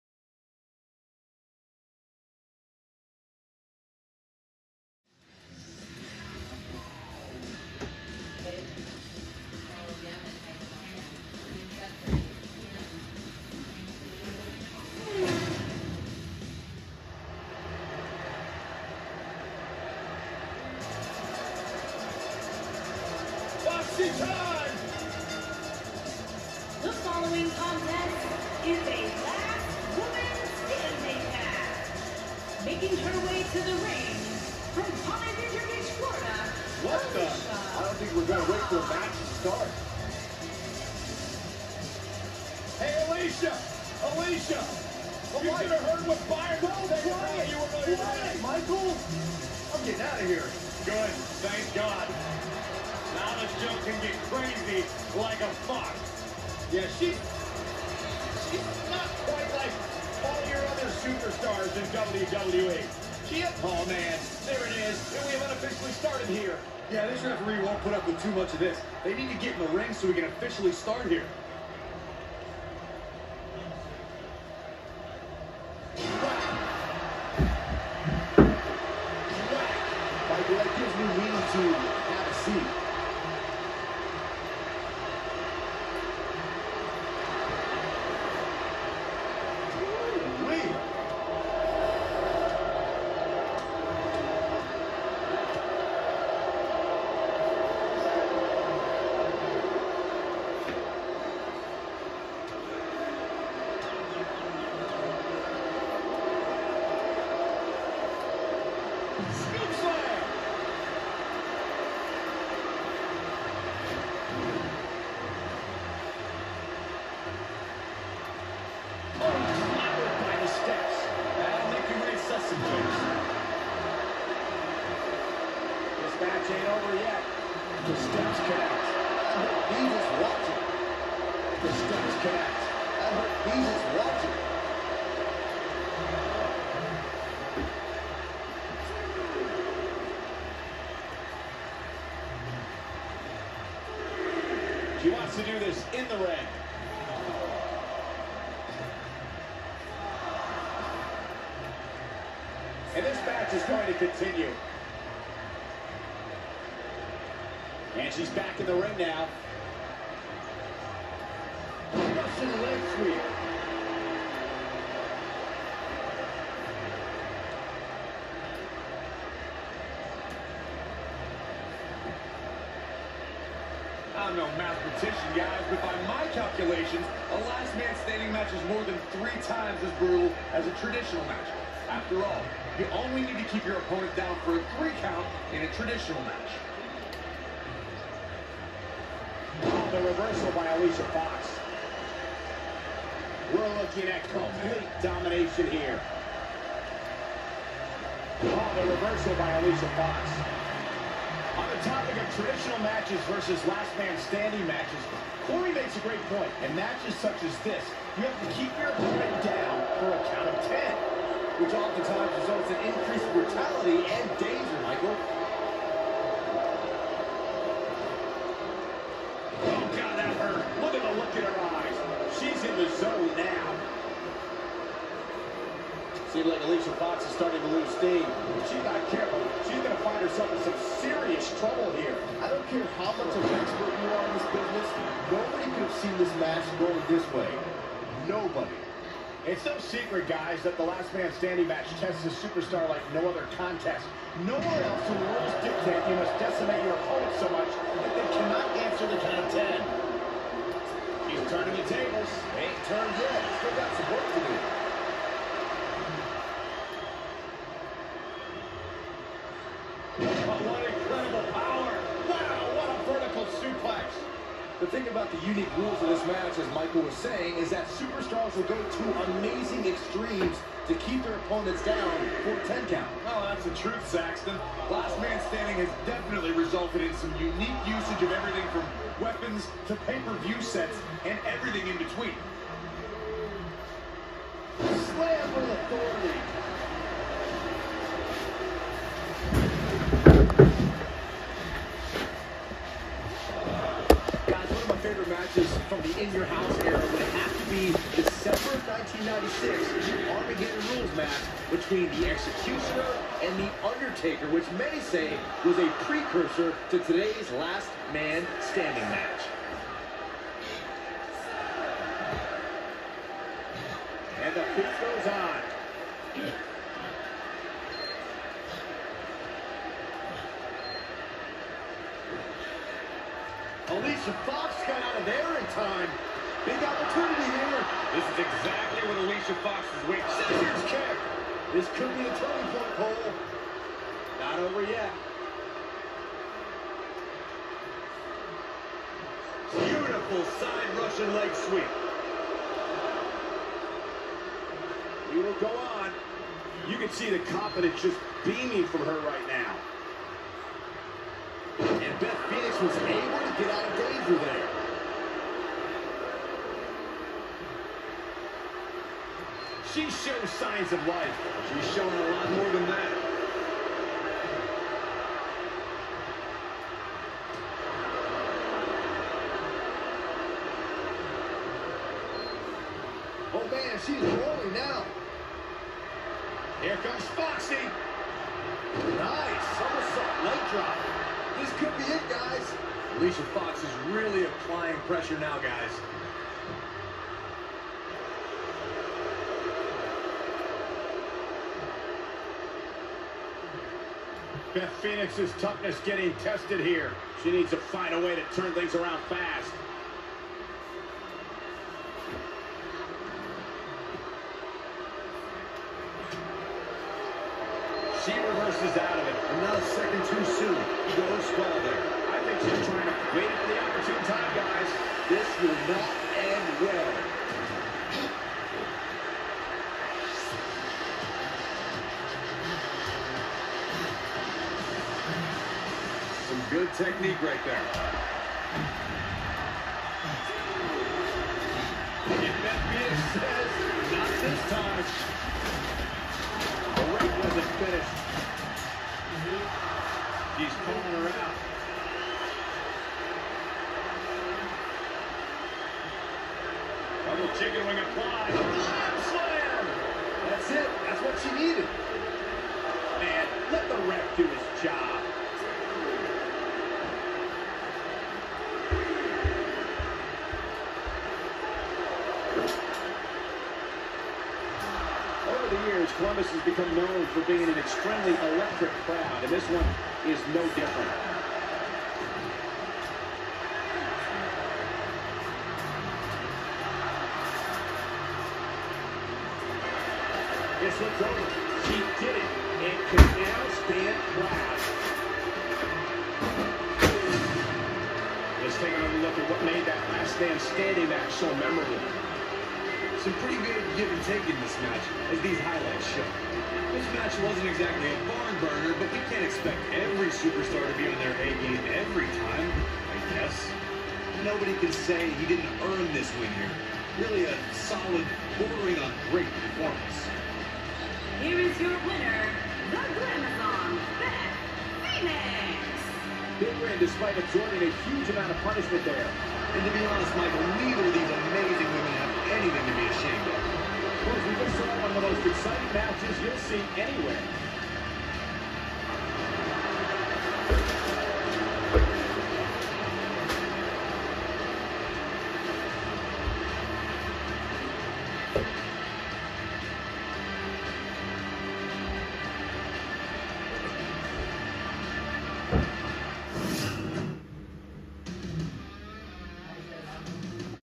Alicia Fox VS Beth Phenoix
Showcasing play through of a Last women Standing Match